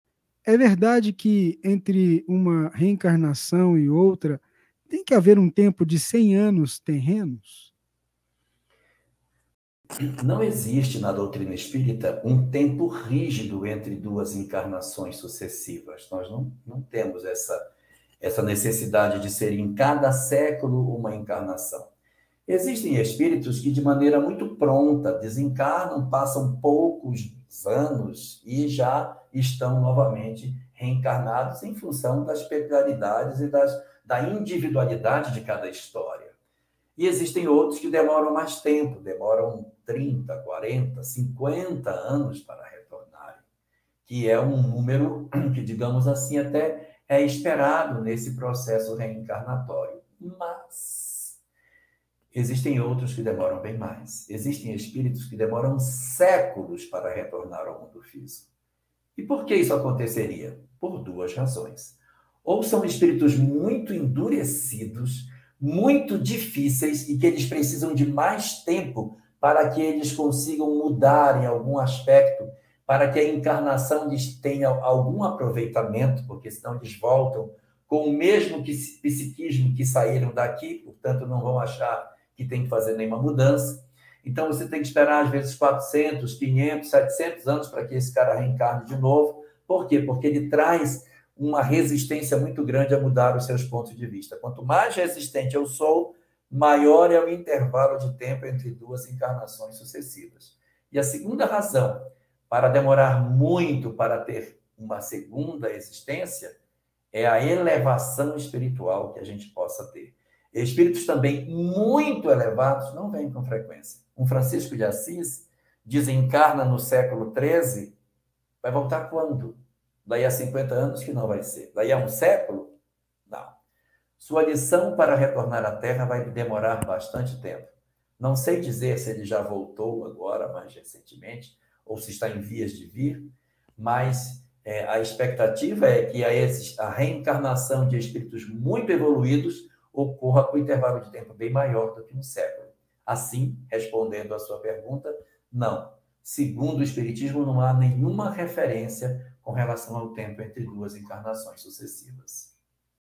É verdade que entre uma reencarnação e outra tem que haver um tempo de 100 anos? Jorge Elarrat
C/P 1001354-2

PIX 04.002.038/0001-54

Gratidão!